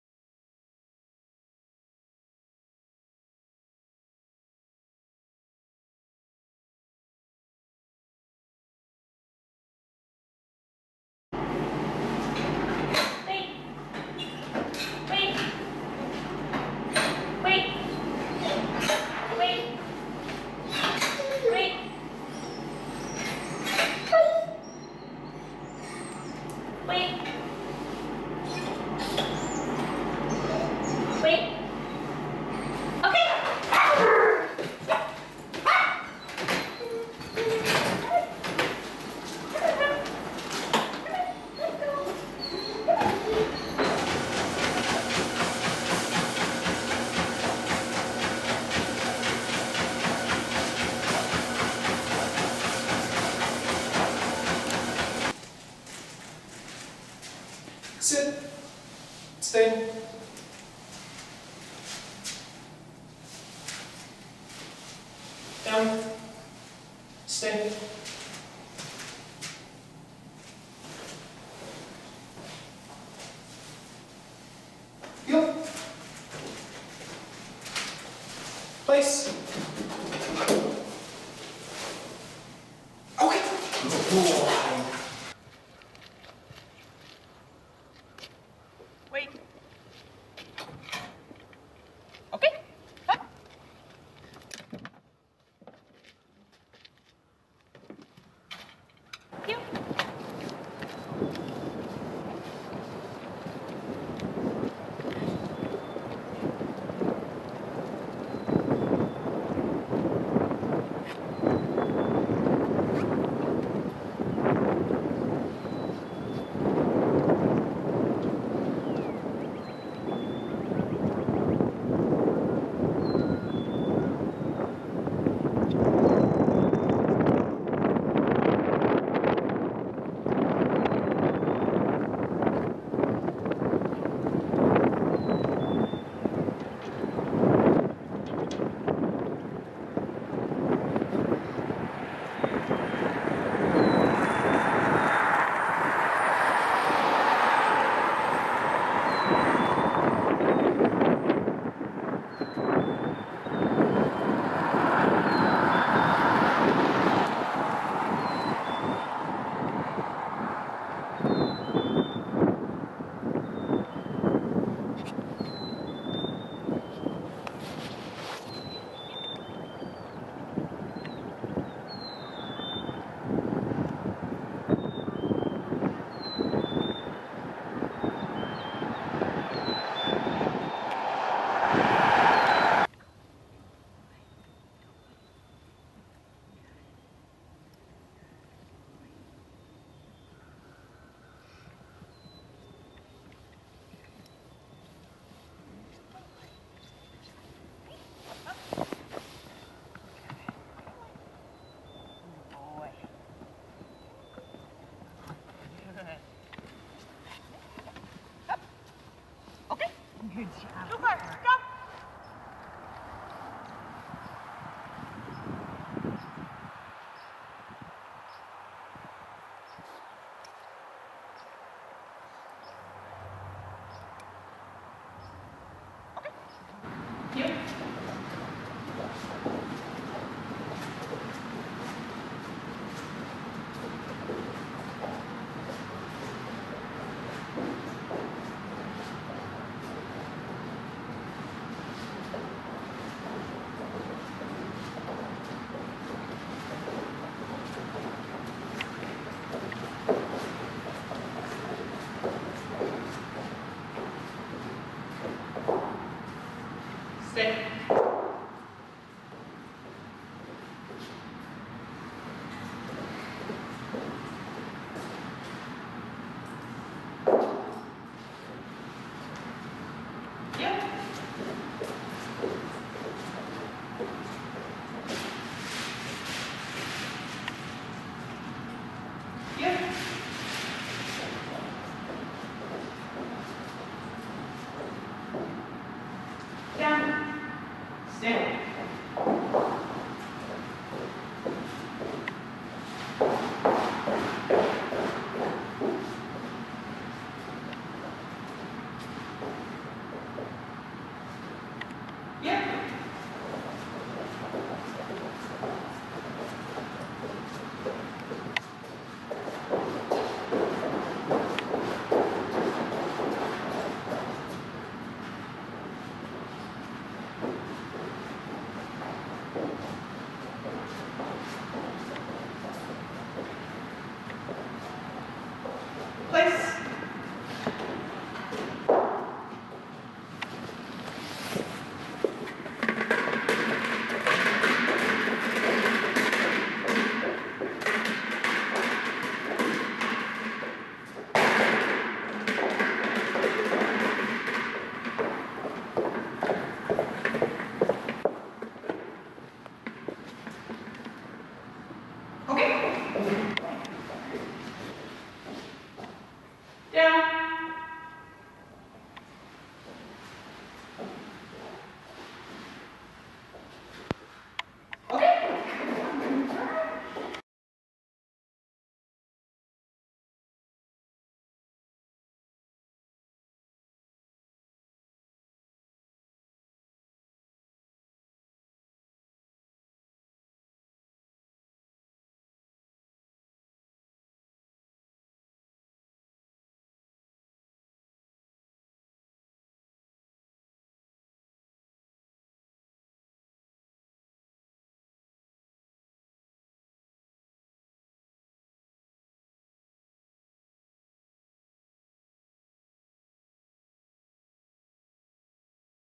Cooper (Mini-Goldendoodle) Trained Dog Demonstration
Cooper (Mini-Goldendoodle) graduated from the dog training boot camp at Neuman K-9 Academy. This program included obedience commands to sit, stay, heel or walk on a loose leash, come when called, proper etiquette, no jumping up, meeting and greeting people under control, and running on a treadmill.

Our dog training camp provides programs for the Mini-Goldendoodle such as boot camp, obedience training, and puppy camp.